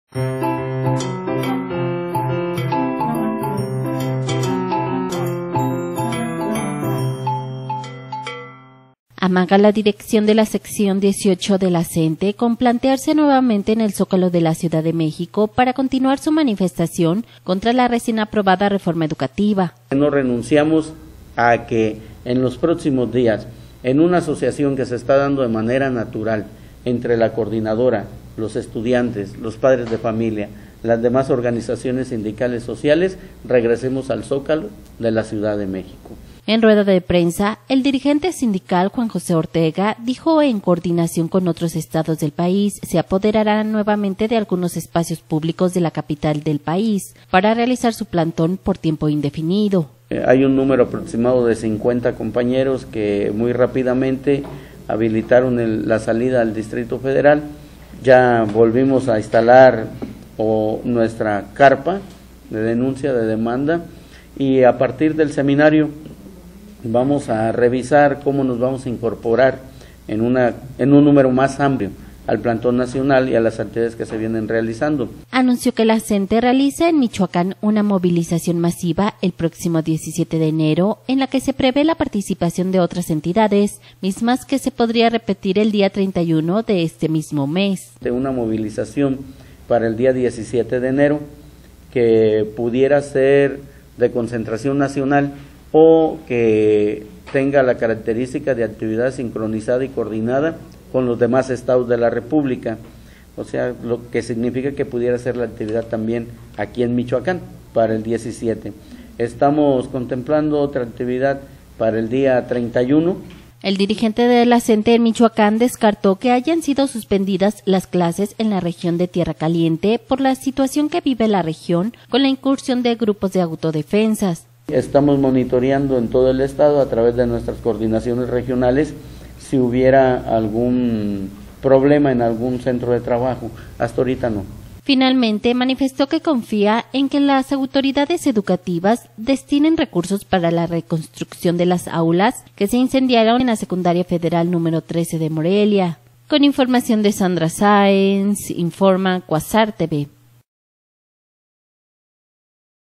Anuncia CNTE que volverá al zócalo en el DF
Amaga la dirigencia de la sección XVIII de la CNTE con plantarse nuevamente en el zócalo de la ciudad de México para continuar su manifestación contra la recién aprobada reforma educativa...